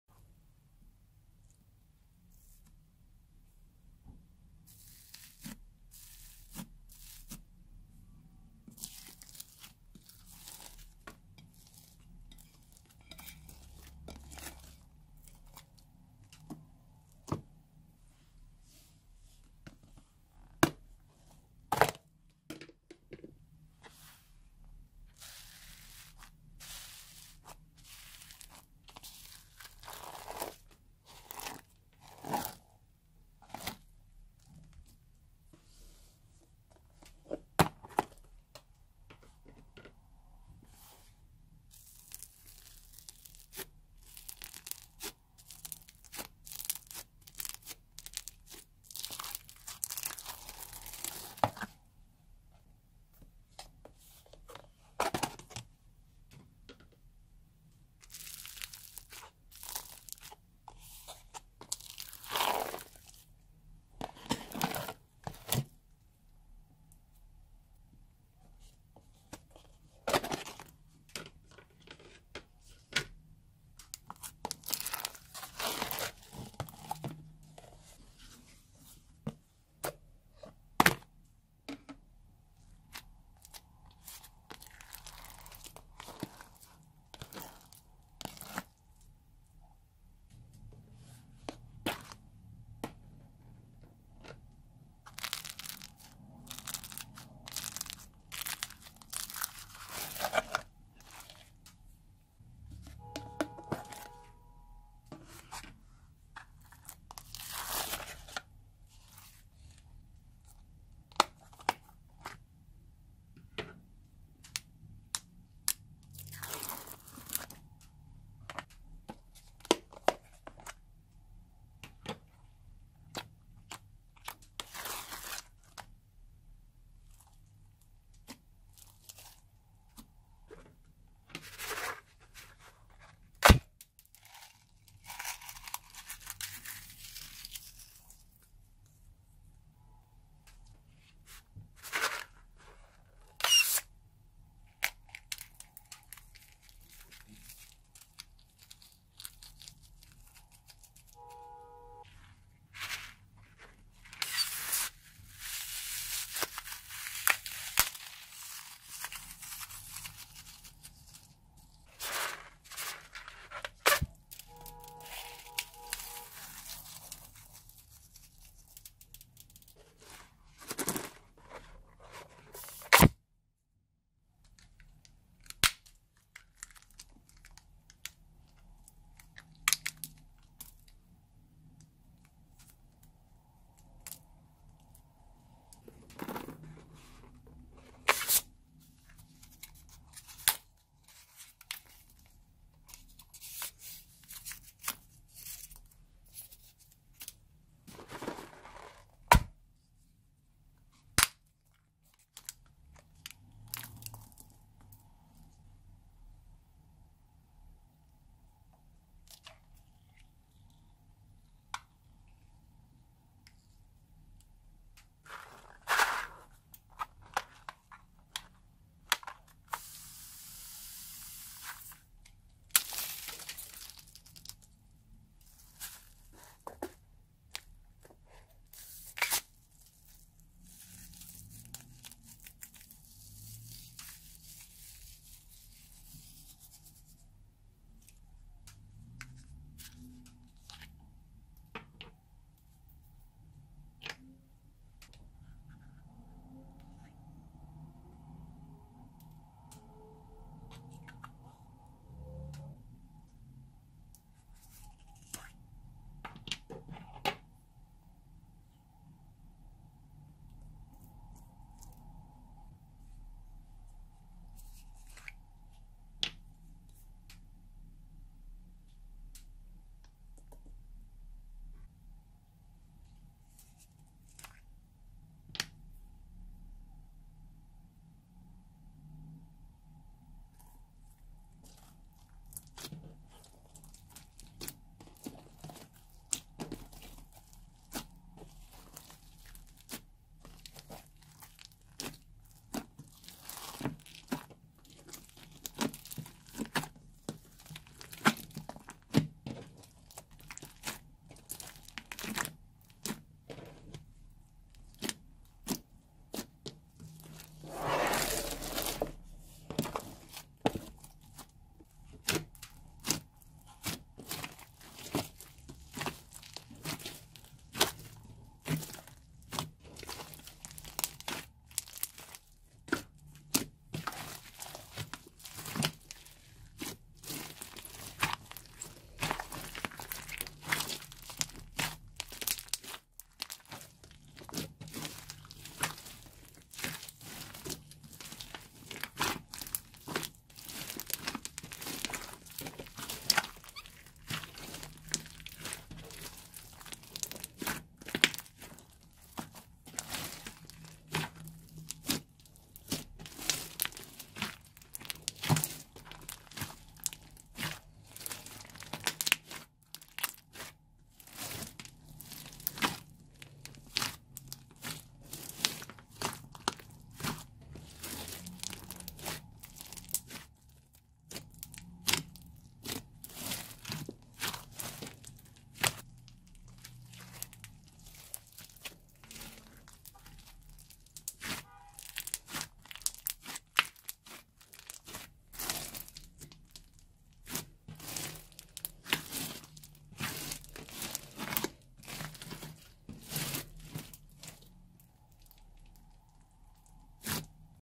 Mixing Old Slime And Stuff From Balloons - Oddly Satisfying
Hi. Today I have mixed some old slime with stuff from balloons. The result is a very crunchy big green slime. Like a swamp.